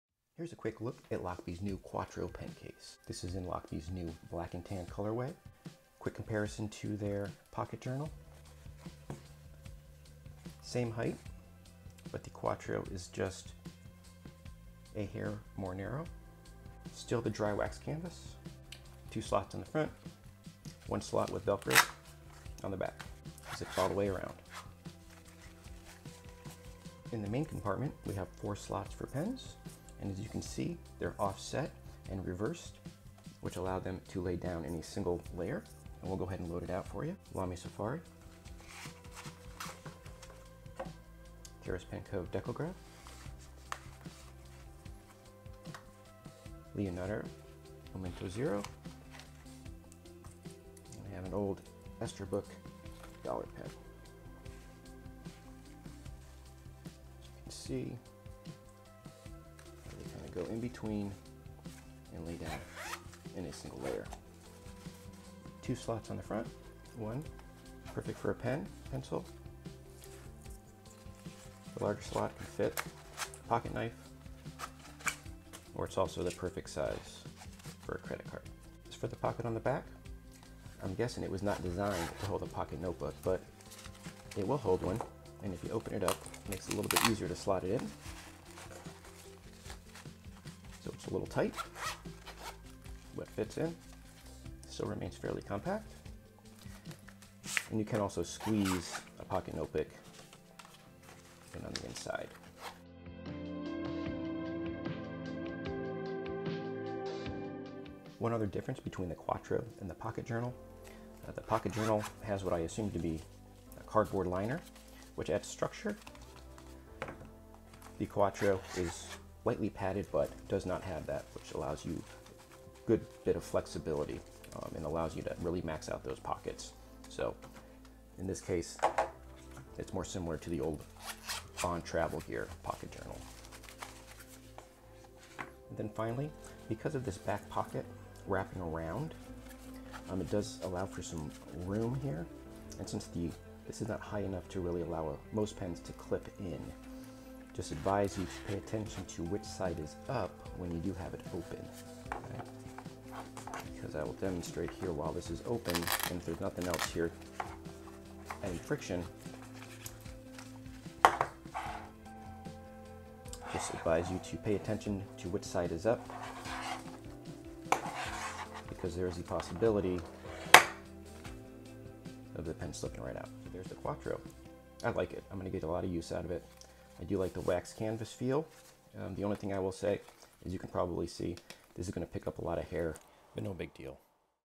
Quick Look at the Lochby Quattro Pen Case
An overview of Lochby's new Quattro pen case. A great, compact edc case wrapped in dry waxed canvas. This case was provided for review by the company.